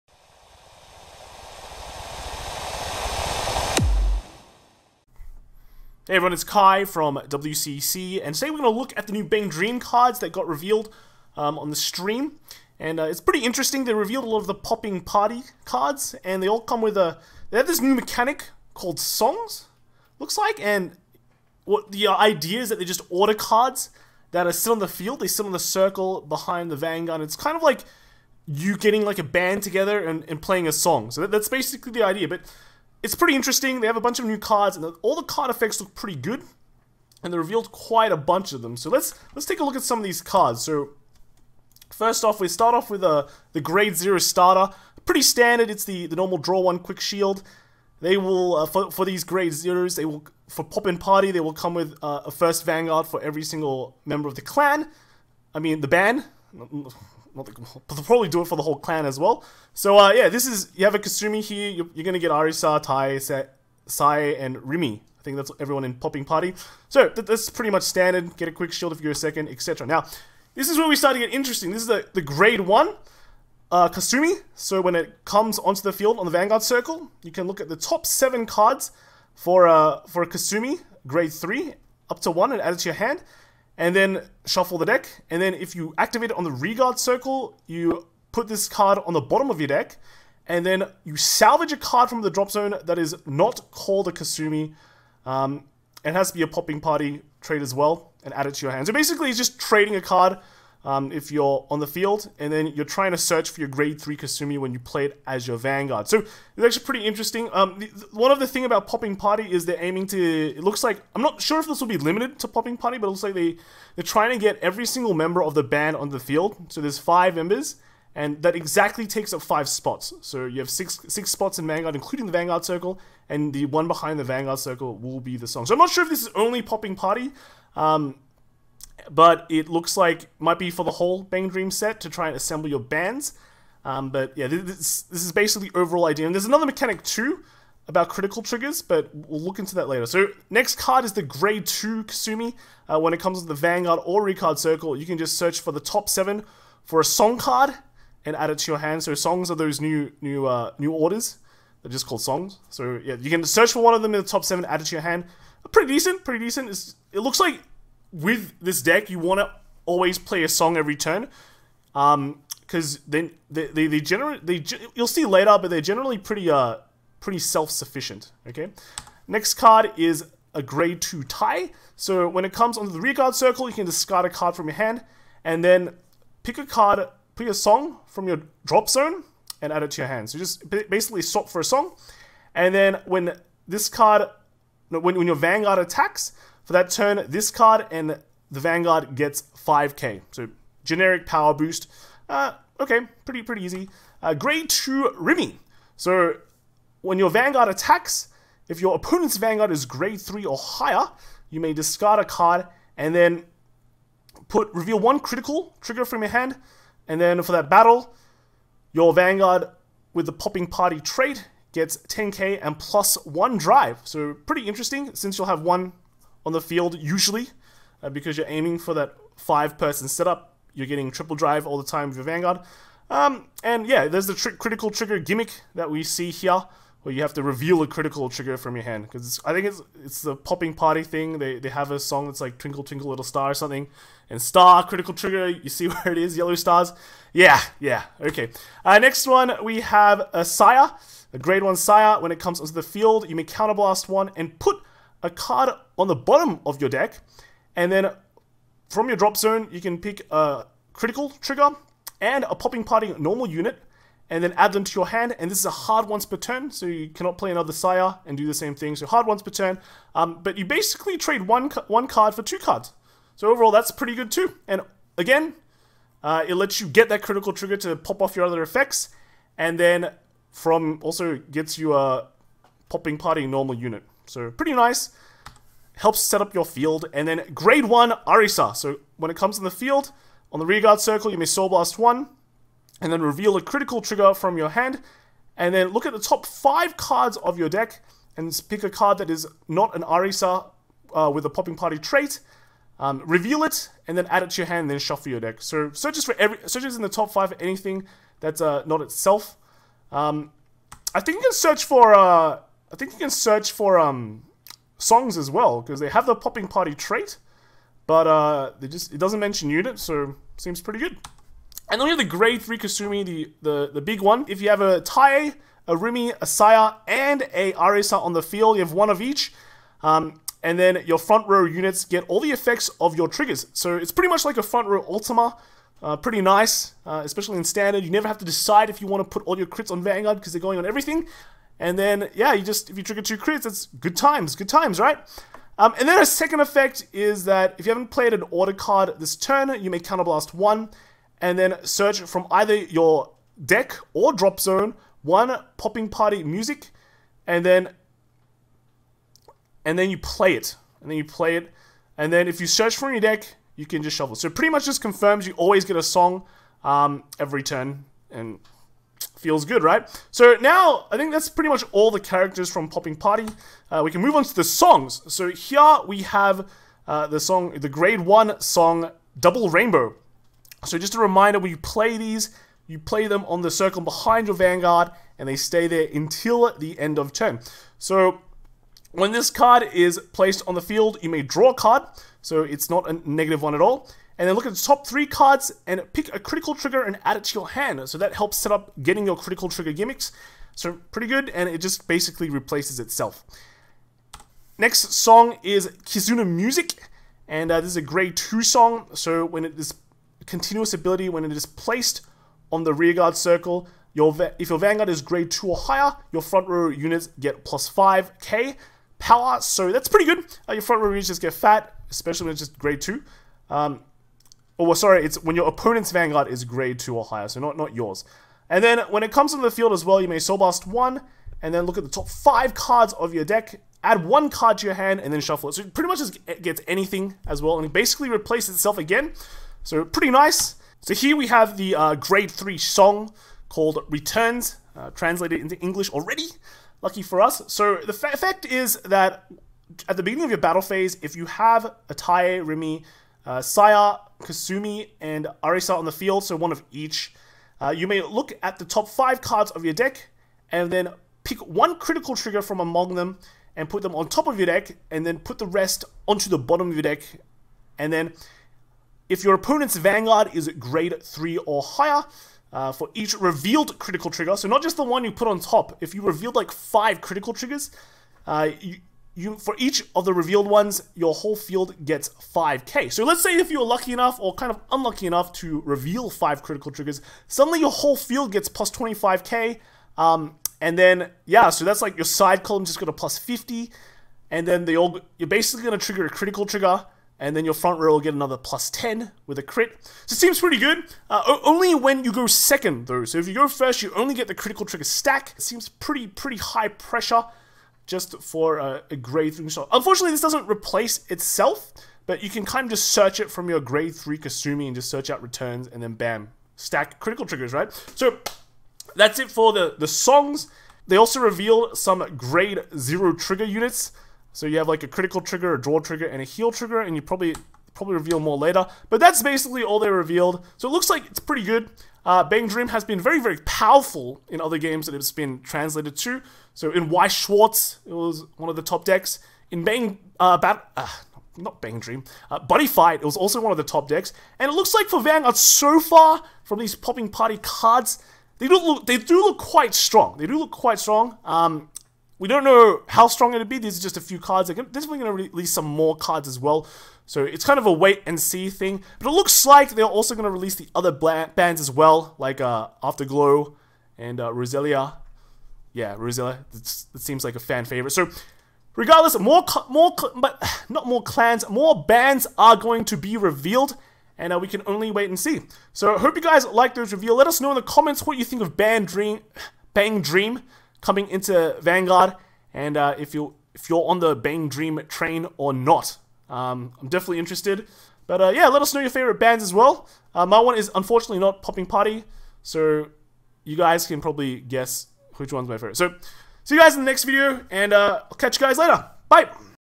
Bang Dream Poppin' Party Cards Revealed! New Mechanic!
Today's stream showed us some new Poppin' Party cards and the new sideways card they've been hinting to us about.